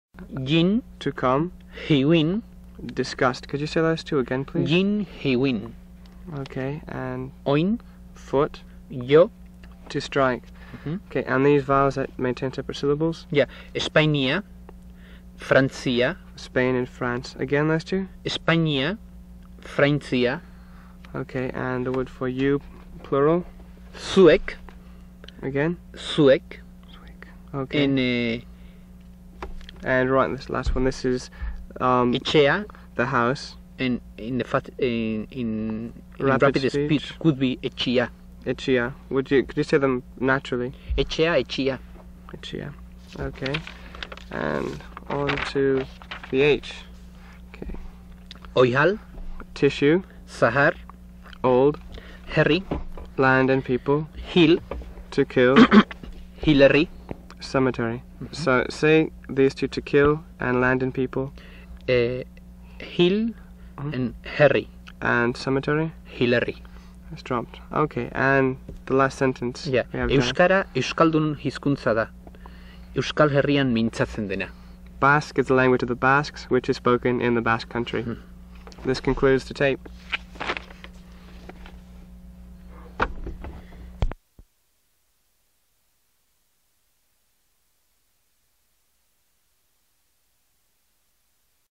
Basque audio: eus_word-list_1983_03
Basque (eus) audio: eus_word-list_1983_03
Language family: Basque
UCLA Phonetics Lab
Date: 5/27/1983

Description: Language Basque.  eus_word-list_1983_01.html, enties: 97 - 109

Contributor: N/A
UCLA Phonetics Archive